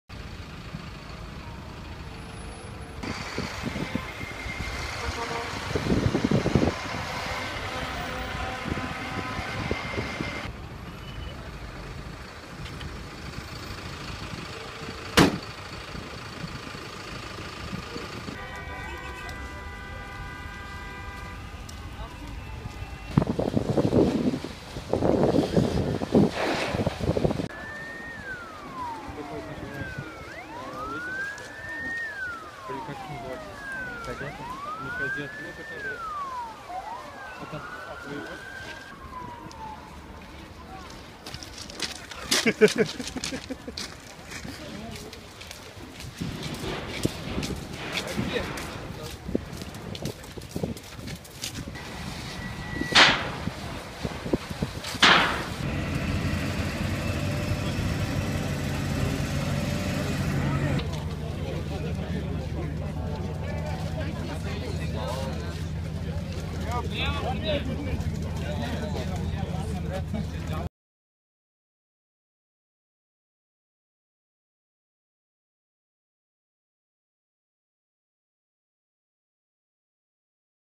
Из-за пожара в ТЦ «Адмирал» пробка растянулась на 3 км
Пробка на Кировской дамбе растянулась на 3 км - об этом свидетельствуют данные Яндекс. Пробки. Источник «БИЗНЕС Online» в ГИБДД Казани подтвердил, что в связи с пожаром в ТЦ «Адмирал» экипажи ДПС перекрыли движение на Кировской дамбе.

Видео: Юлиана Семенова